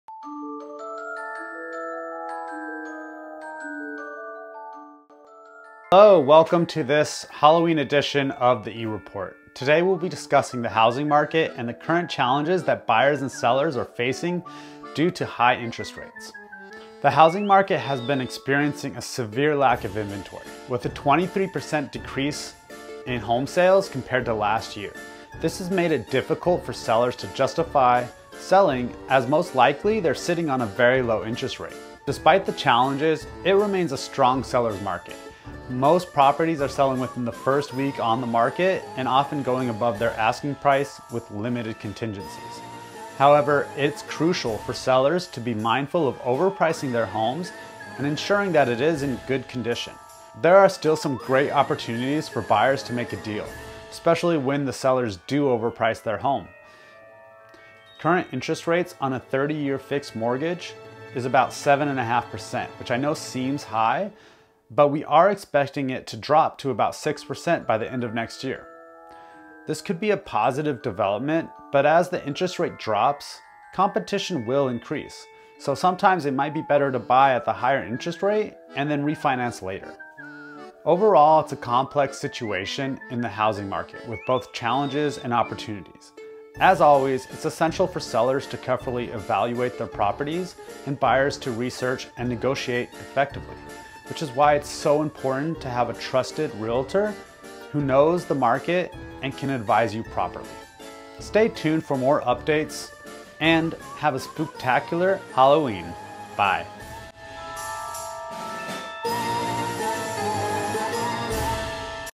Halloween Housing Market!
Hello everyone! In today's video, I dive into the latest trends and updates in the DC Metro Real Estate market for 2023. From sales volume to mortgage rates, get a quick snapshot of what's happening and what we might expect in the near future. Whether you're looking to buy, sell, or just staying informed, this update has got you covered. 💼🏠✨